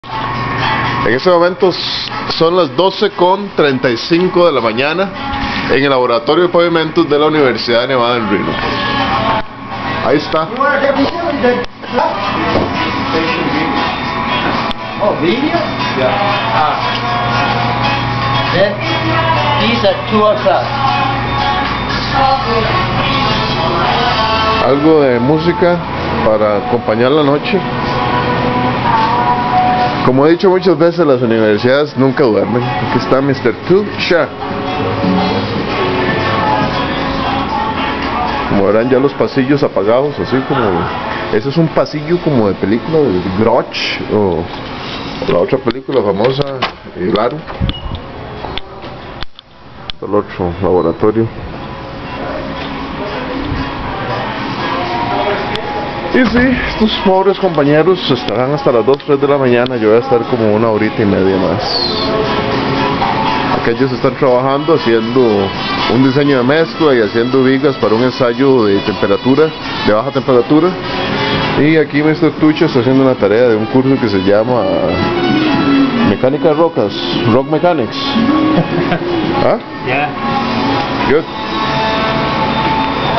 Trabajo nocturno WRSC
Trabajo tarde en la noche en el laboratorio